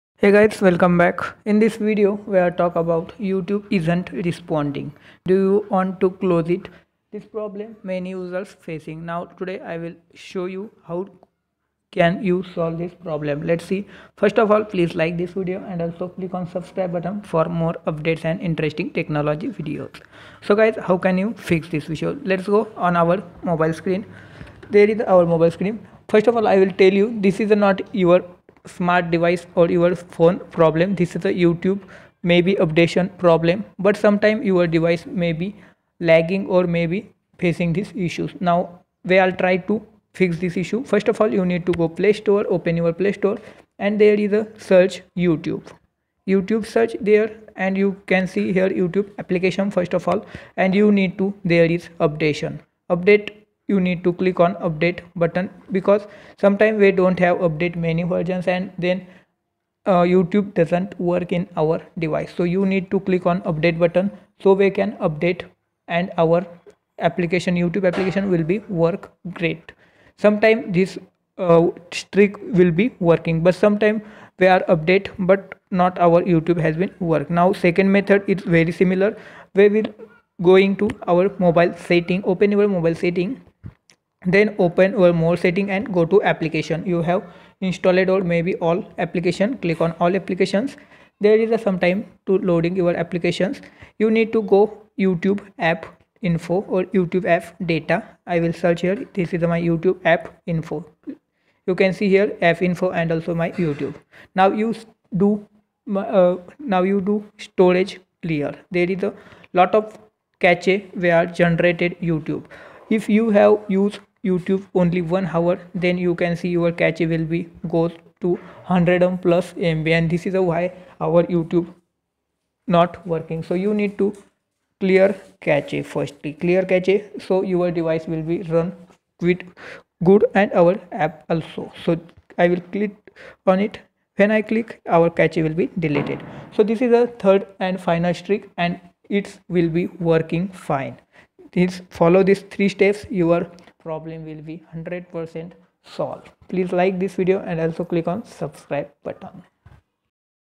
YouTube || YouTube isn't responding. do you want to close it? Problem fix
dosto is video me hum batane wale hai YouTube isn't responding. do you want to close it? Problem Solve

Channel Owner - Vaibhav Gosavi
Video Editor - Video Guru App
Screen Recorder - X Recorder App